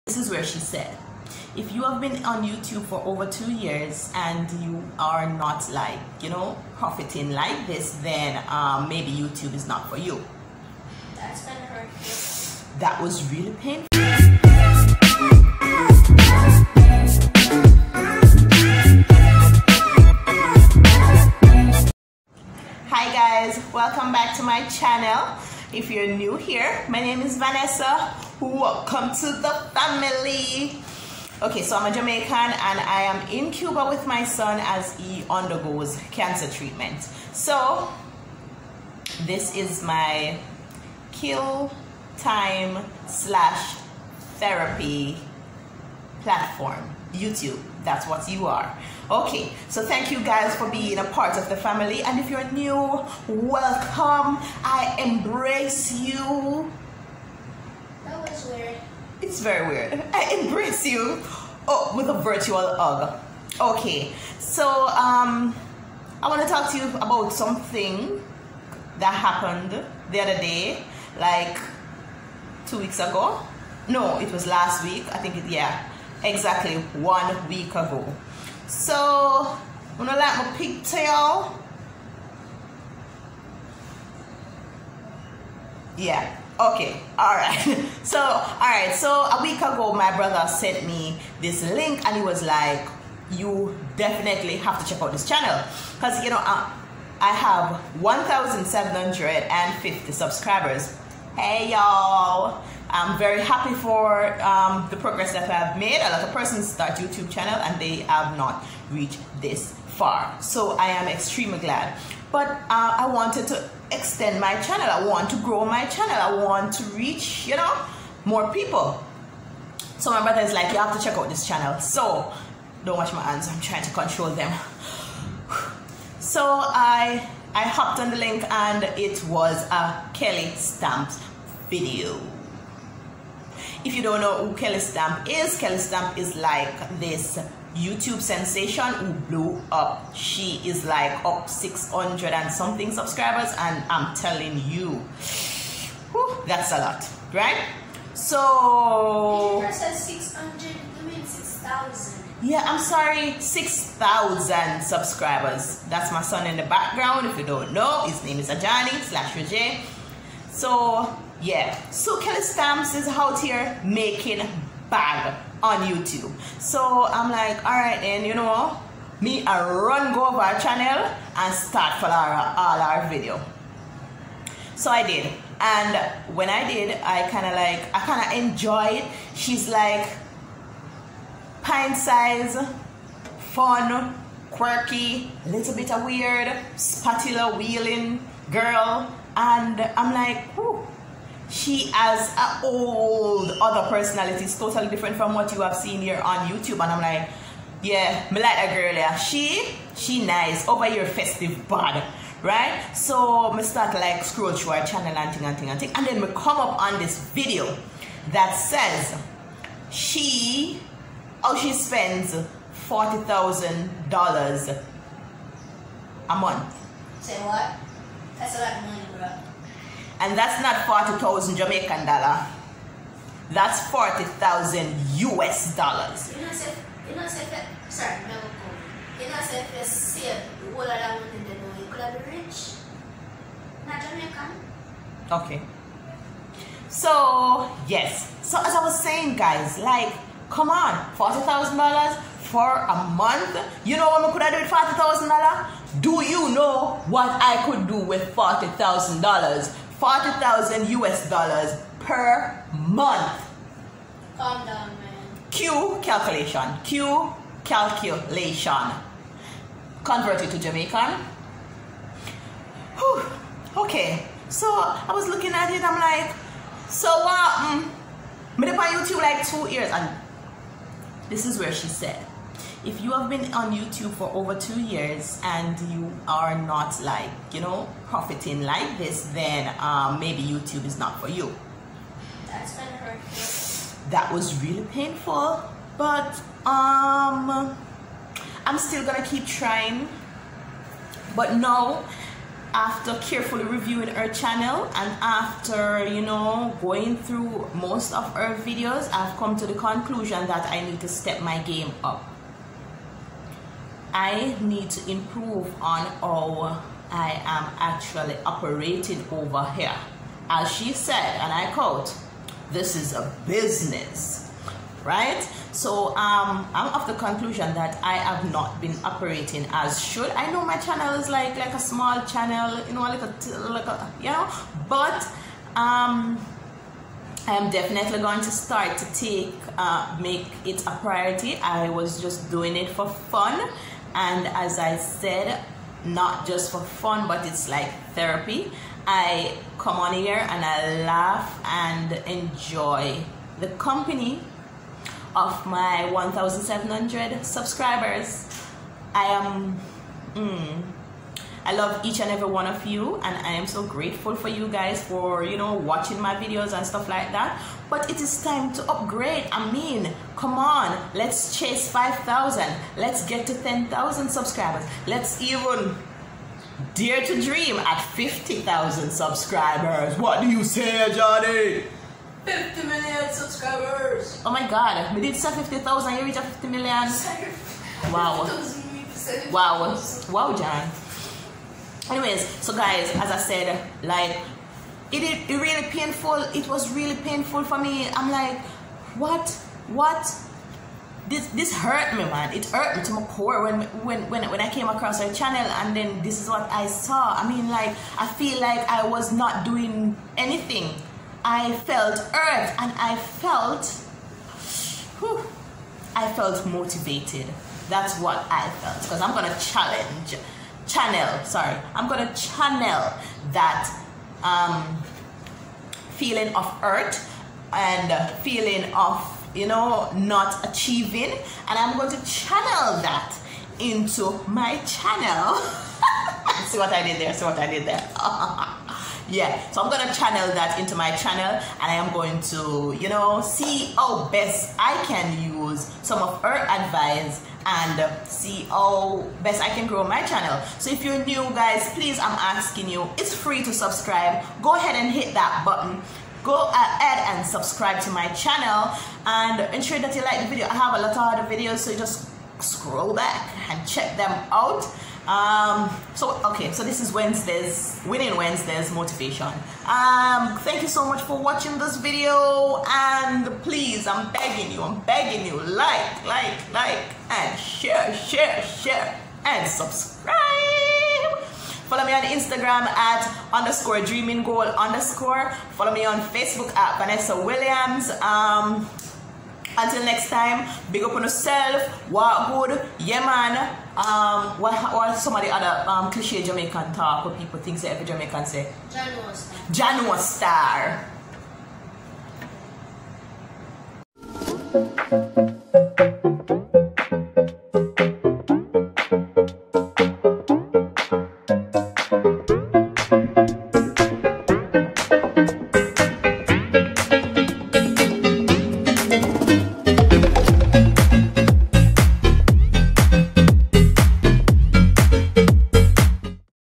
KELLY STAMPS MADE 40K FROM YOUTUBE PER MONTH
One of the most influential Youtuber and overnight sensation Kelly Stamps had some pretty interesting things to say in one of her most recent videos. She earn an income of over 40k per month from her YouTube channel.